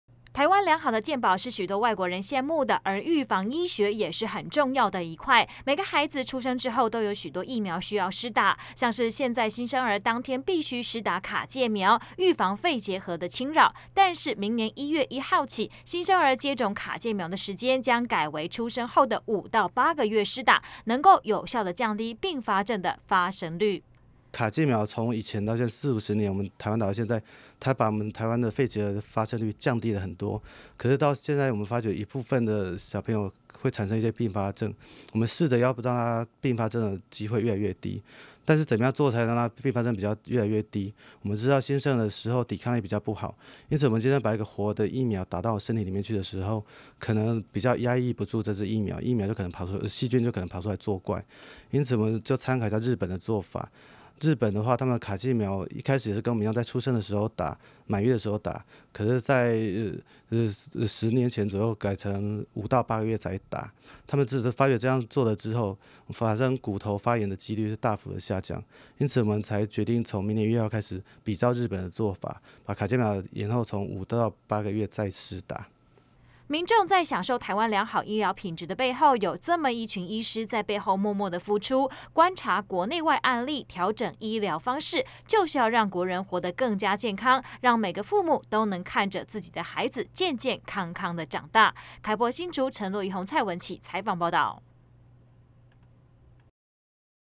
20151210N 卡介苗改變施打時間 明年全台實施
卡介苗是預防肺結核很重要的一種措施，目前台灣是幼兒出生當天必須要施打的一種疫苗，但明年一月一號起，幼兒疫苗接種的時間改為出生後的五到八個月，透過延後接種，能有效降低併發症的發生。